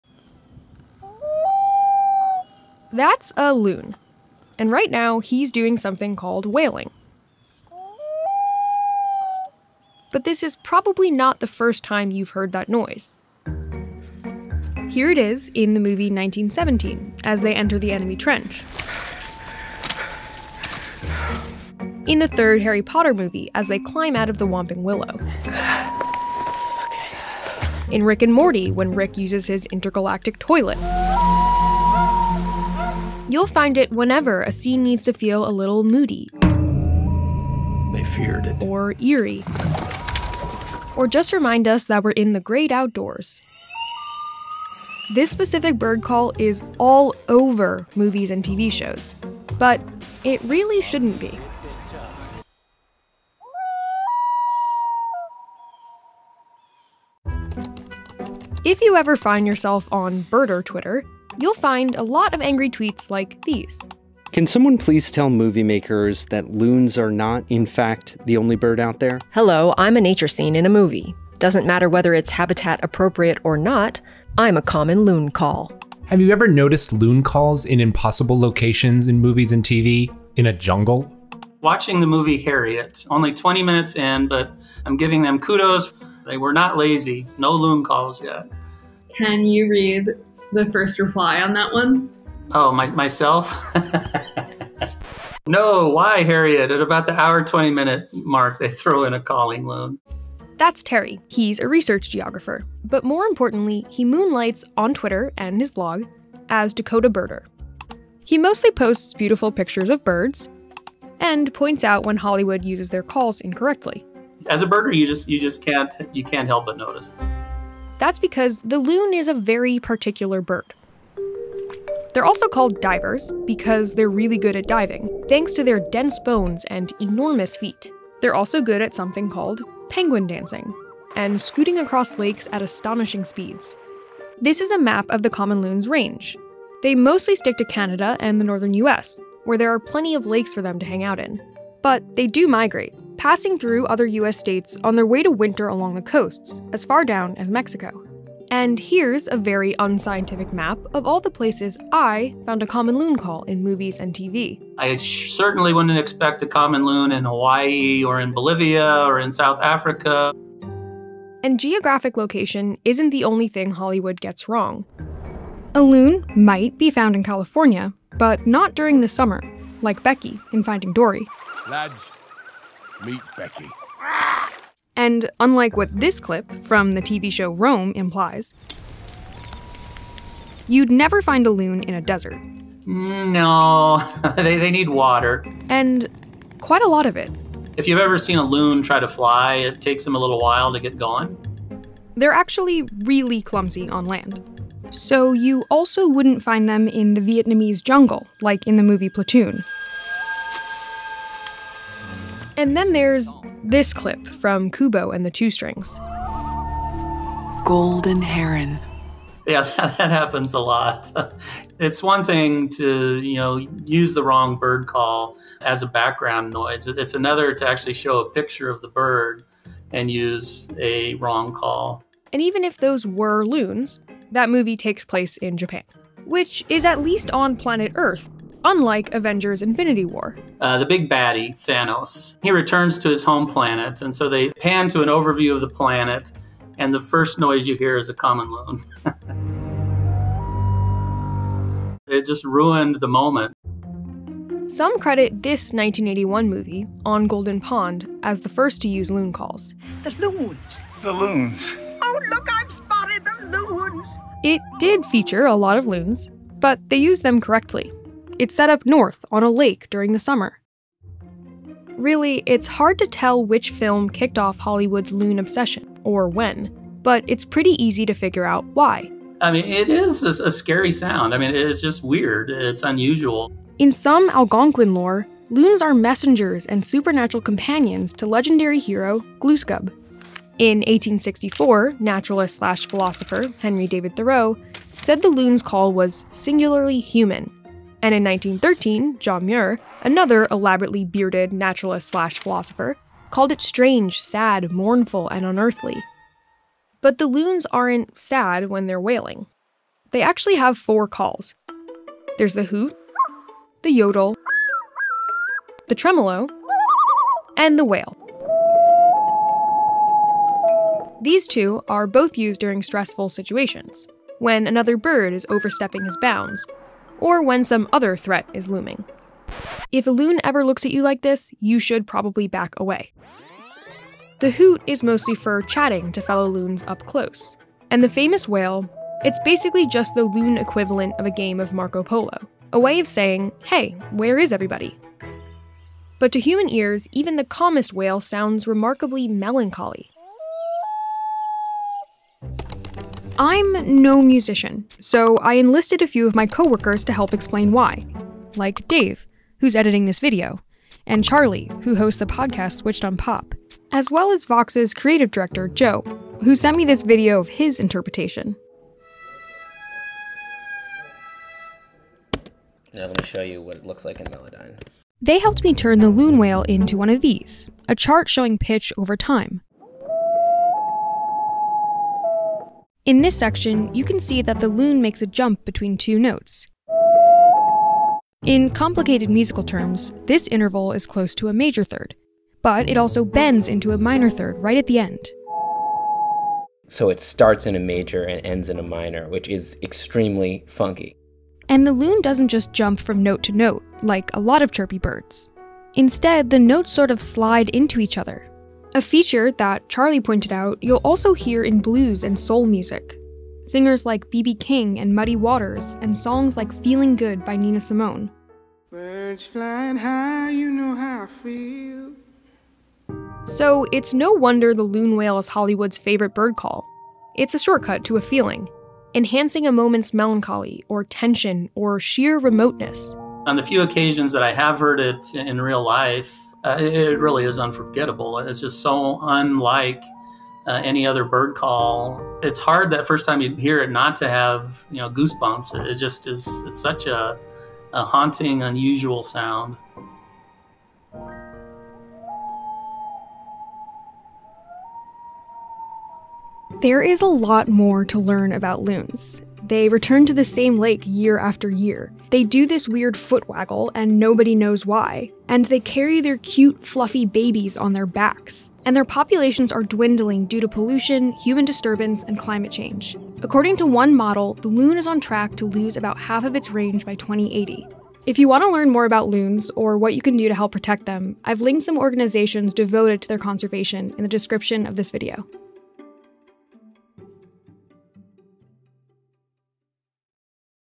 Why Hollywood loves this creepy bird call
Once you hear it, you can’t unhear it. Sorry!

If you’ve seen any remotely creepy movie, you’ve heard a loon wail: that mournful, nearly human ooo-ooo-oooo. It pops up whenever a movie needs to convey melancholy or trepidation, often accompanied by a full moon or some fog. But for the most part, loons have no business being in the background of those scenes.

With the help of some internet birders, we took a deep dive (pun intended) into the world of loons to explain how Hollywood misuses this bird call — and why the wail is so haunting in the first place.

To learn more about loons and what you can do to help protect them check out the Loon Preservation Committee and the Adirondack Center for Loon Conservation

Check out the Switched on Pop podcast from Charlie Harding